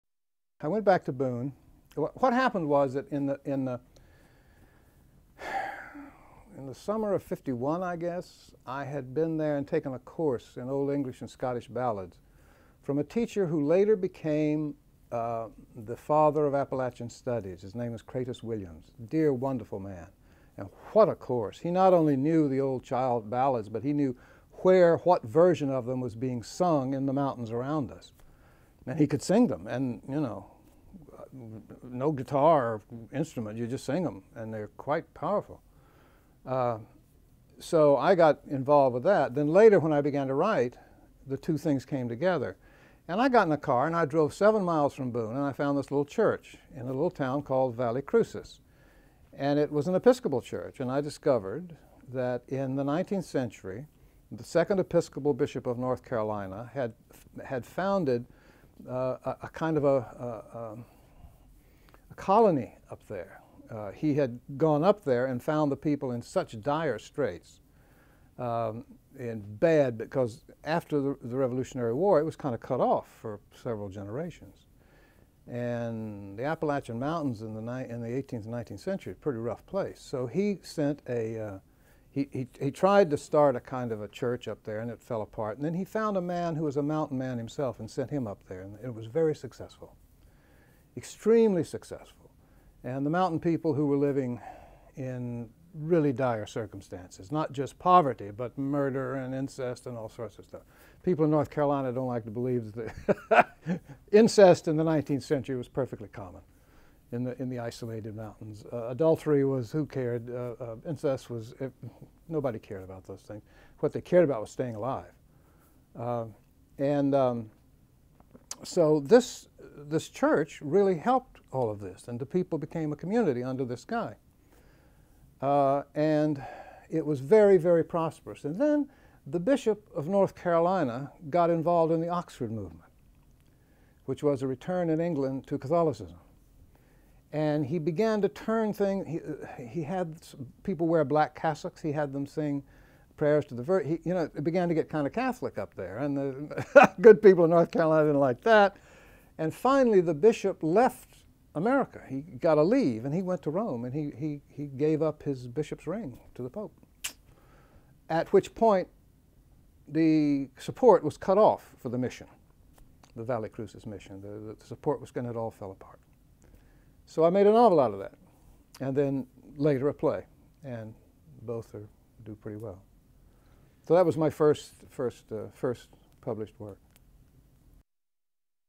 Romulus Linney: Heathen Valley
Playwright Romulus Linney IV (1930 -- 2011) was raised in Boone, North Carolina and Madison, Tennessee. Many of his plays have Appalachian settings (Tennessee, Holy Ghosts, Sand Mountain, Gint, and Heathen Valley) while others focus on European and military themes (The Sorrows of Frederick, 2, The Love Suicides at the Schofield Barracks. Childe Byron is his romantic play about Lord Byron.  Linney received a BA from Oberlin College and an MFA from the Yale School of Drama.  Romulus Linney was interviewed by Mike Wood on January 14, 2003 at Linney's home in Germantown, NY.  The interview segments are courtesy of the William Inge Center for the Arts in Independence, Kansas.